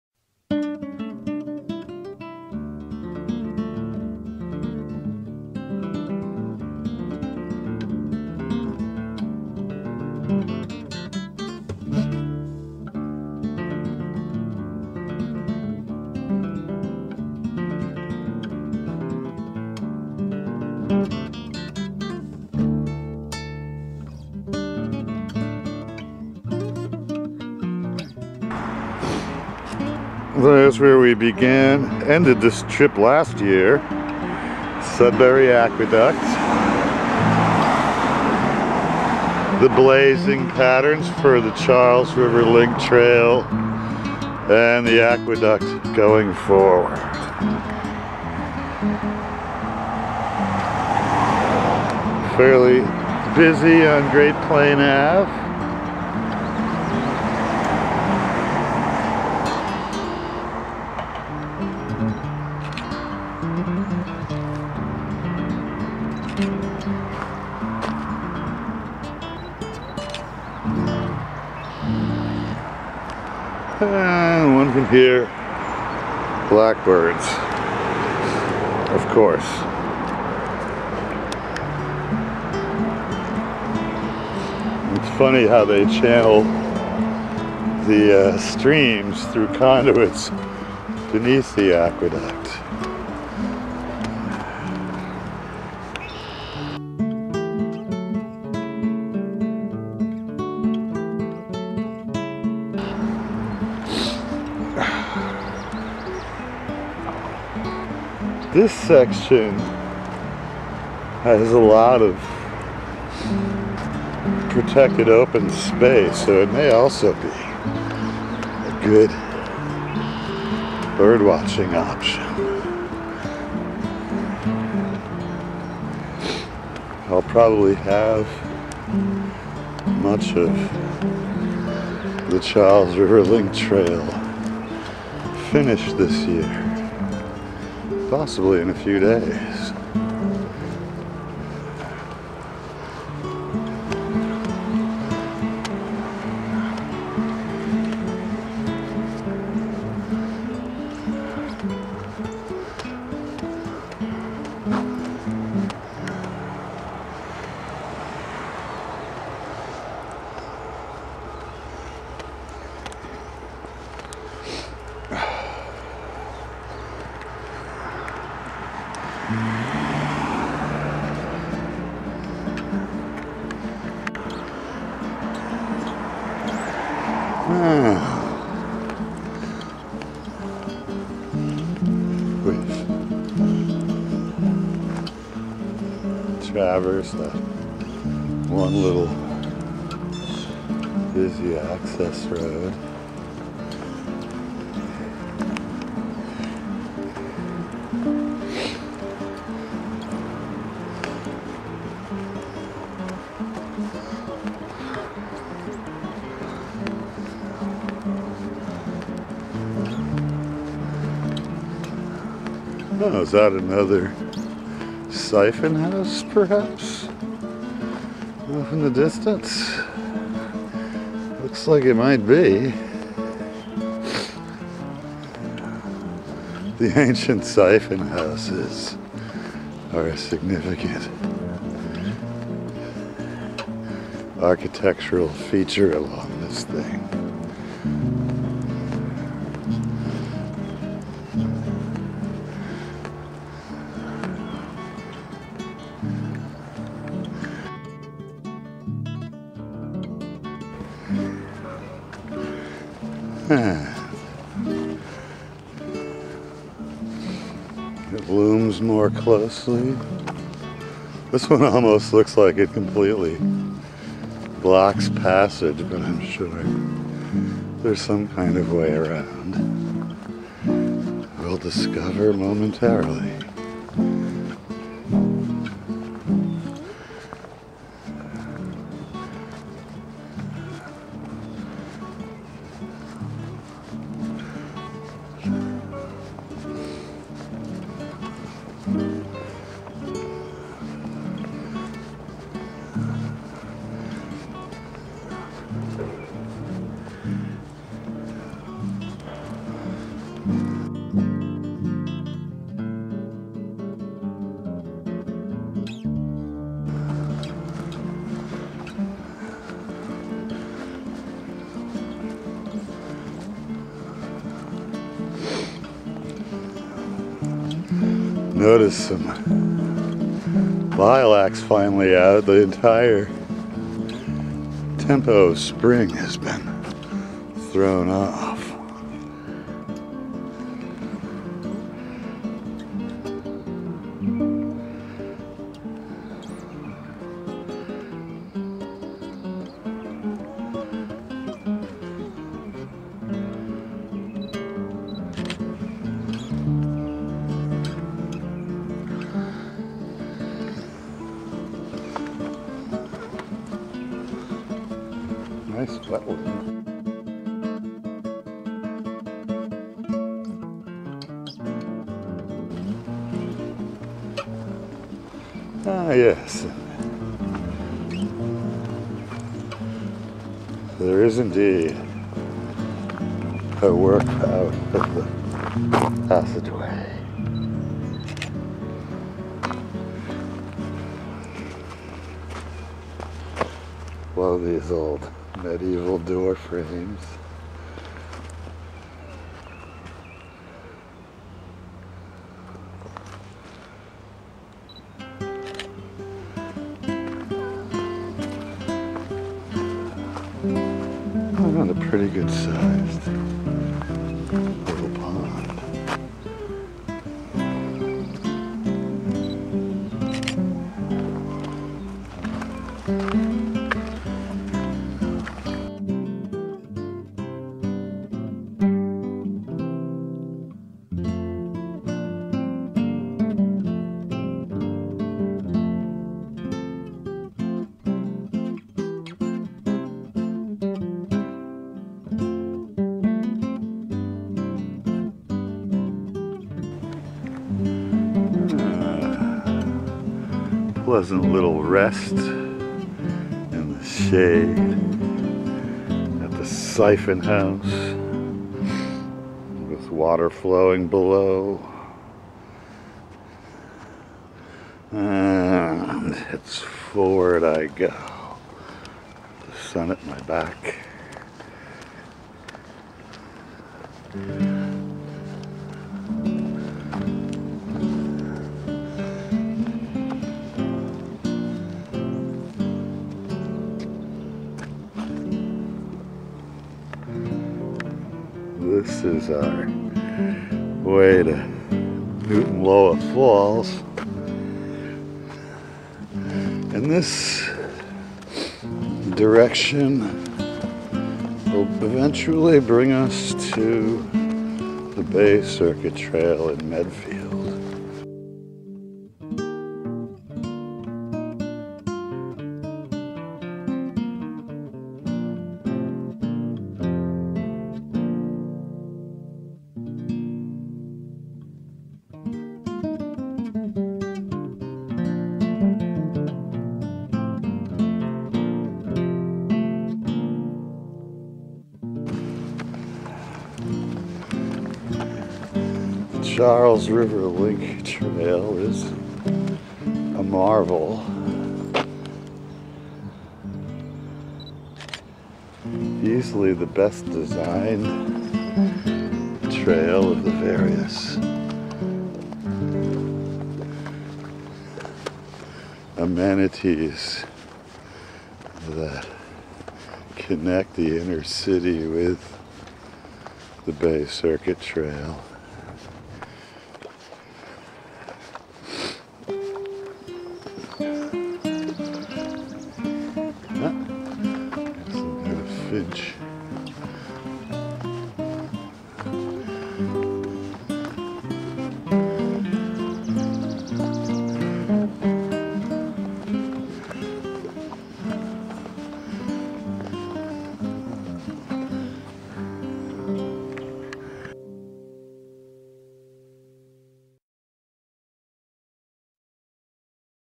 Charles River Link Trail Part 14. Return to the Aqueduct.
The Charles River Link Trail continues along the Sudbury Aqueduct after it crosses Great Plain Ave.